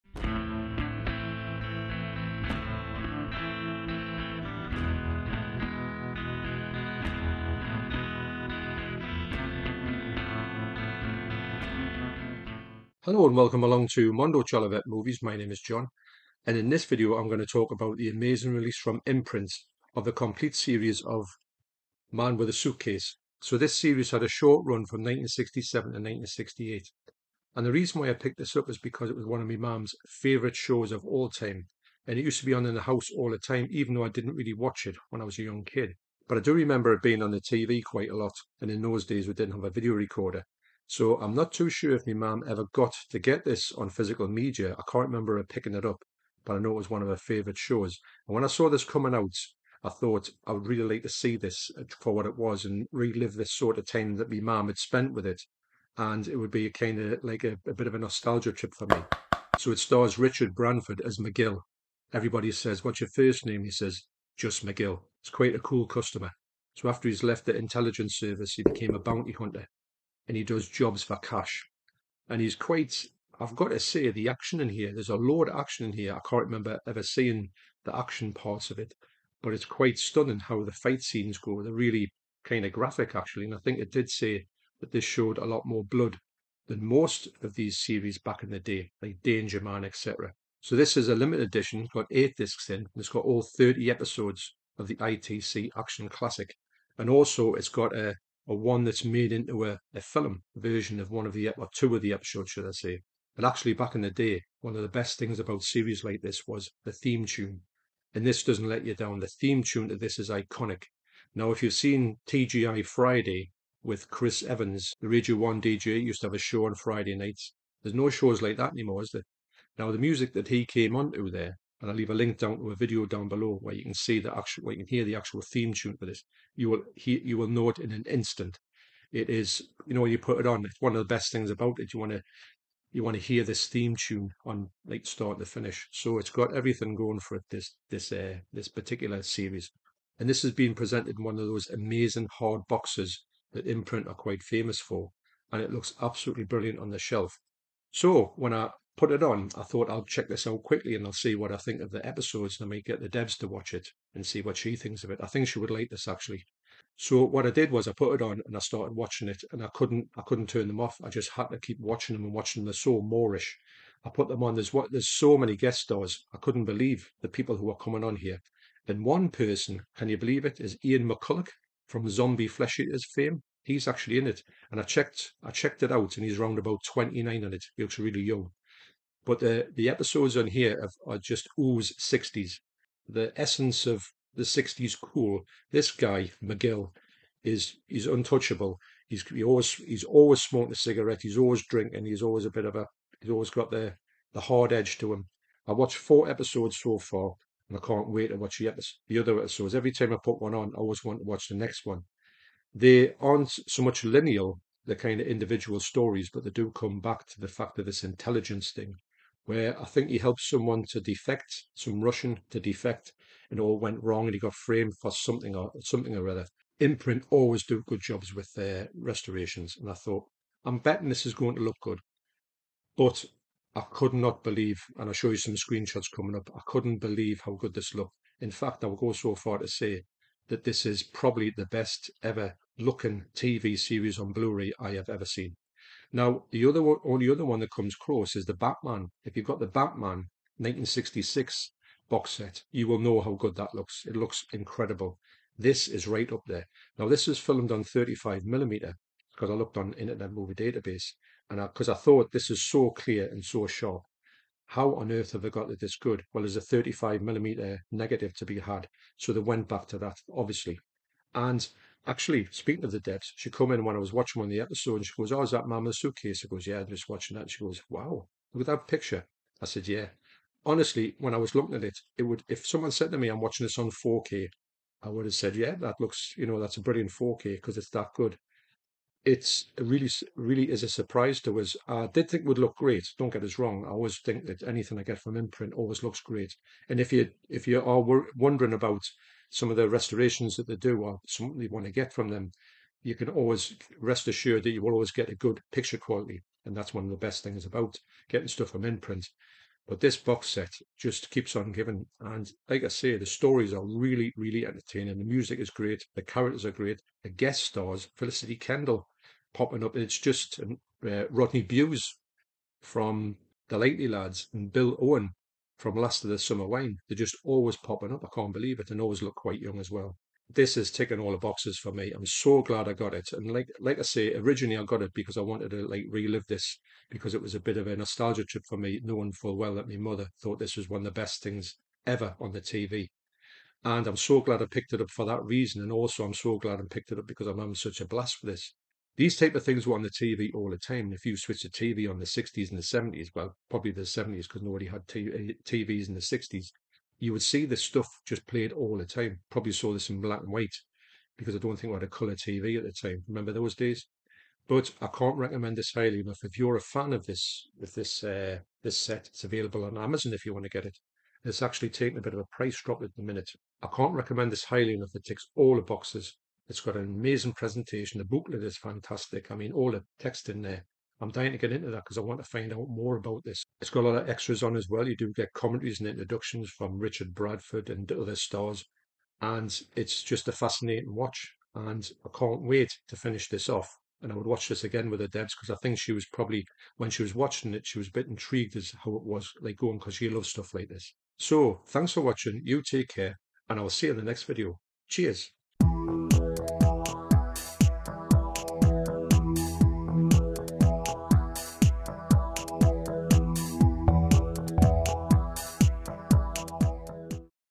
Stunning boxset from Imprint Releasing
Here is a video on how great this box I've recieved from Imprint Releasing in Australia.